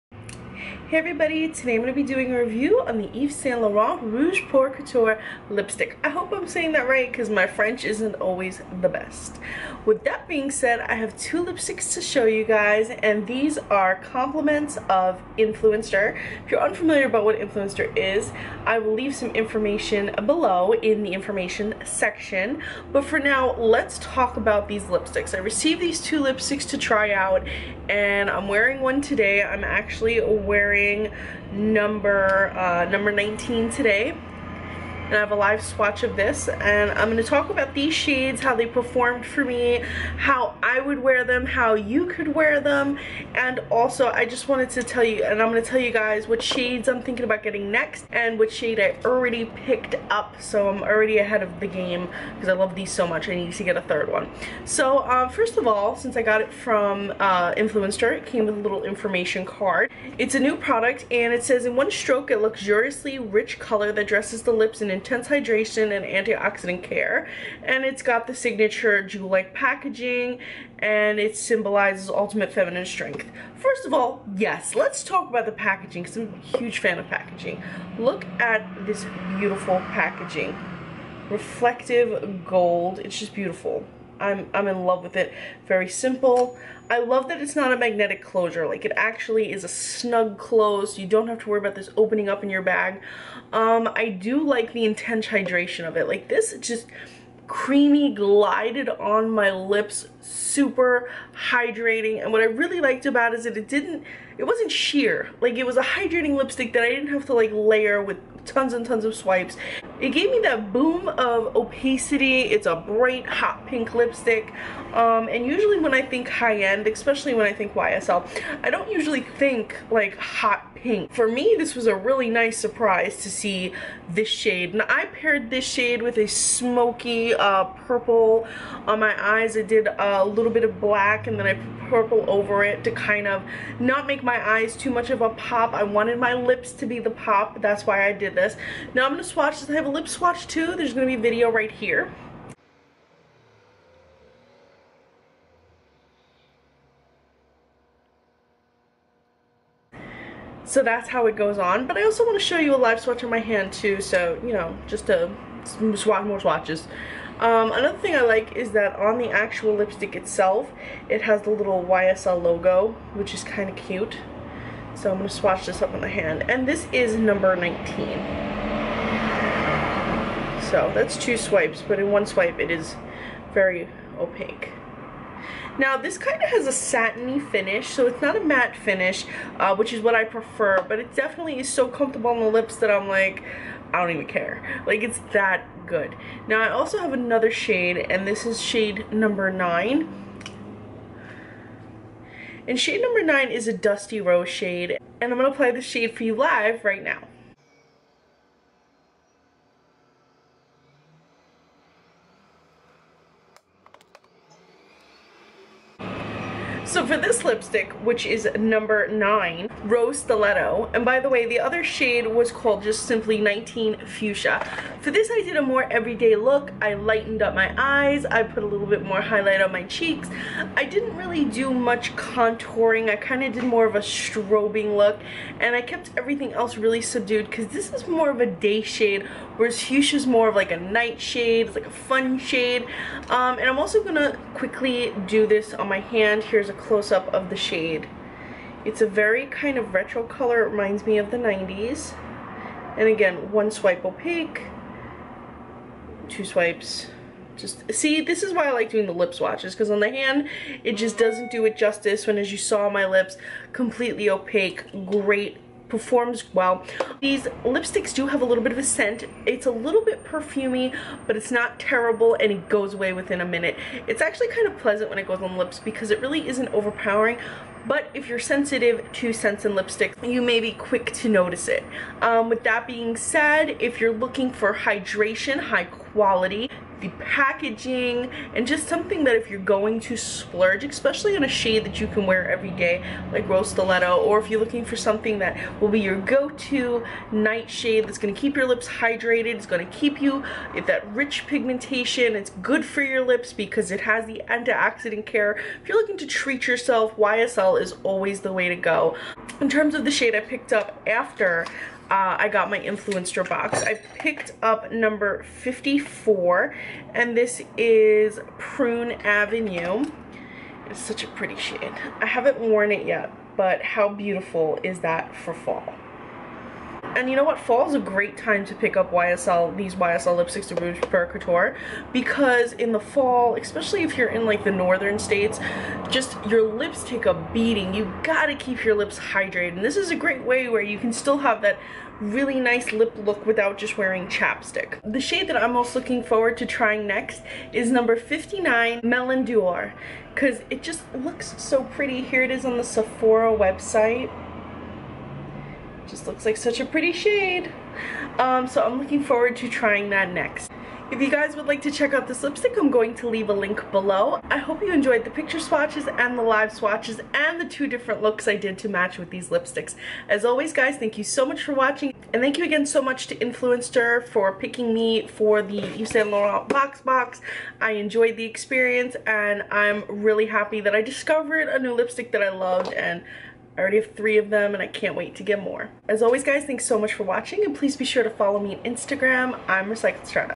Yves Saint Laurent Rouge Pur Couture Lipsticks Review Swatches
I received this complimentary from influenster for reviewing purposes.

Shades featured:
Rose stiletto
Fushia